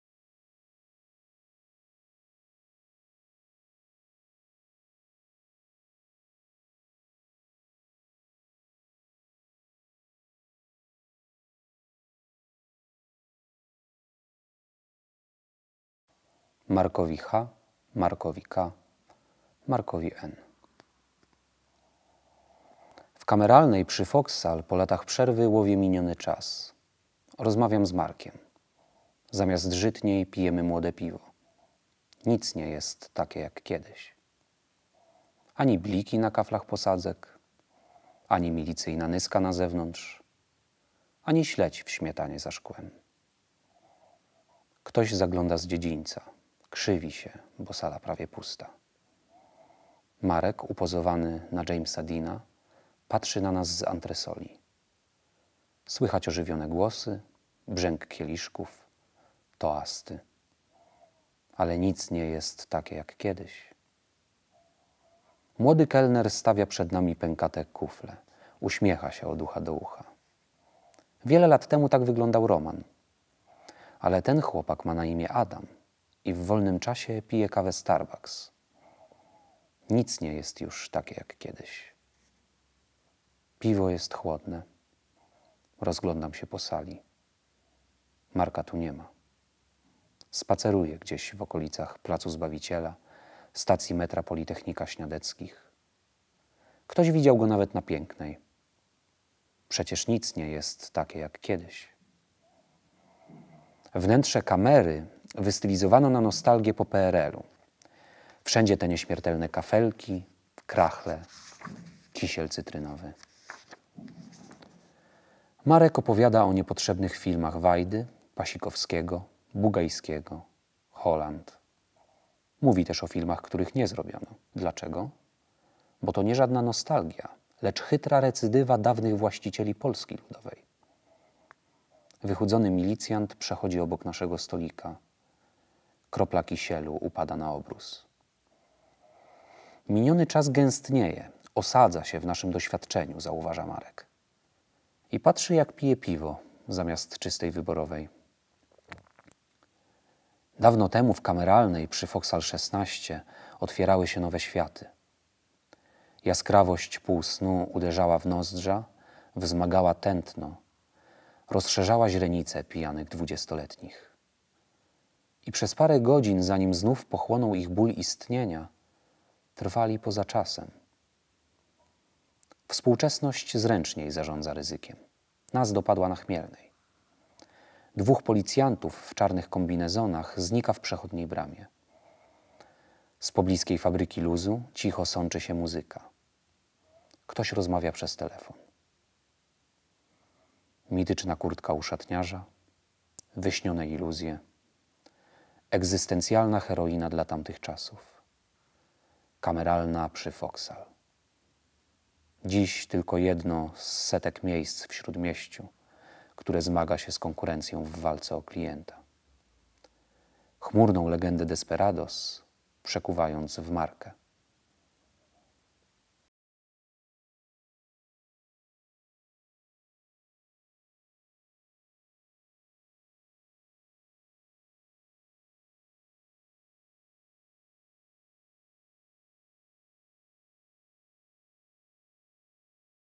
Waldemar Żyszkiewicz "Kameralna Foksal 11" czyta Adrian Zaremba
realizacja :
Olga Czyżykiewicz
operator:
Adam Romanowski
dźwięk:
Janusz Dąbkiewicz
montaż:
Magdalena Pogoda
kierownik produkcji:
Łukasz Borkowski